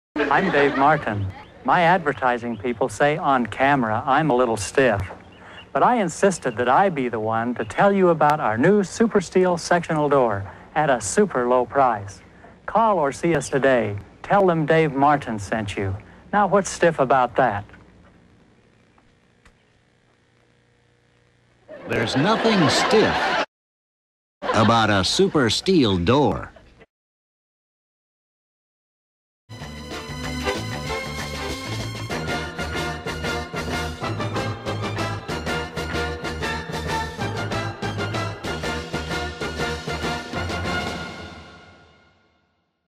Cool Super-Steel Garage Door Commercial - from 1994!
Taped in late 1994, here is a cool commercial for super-steel garage doors!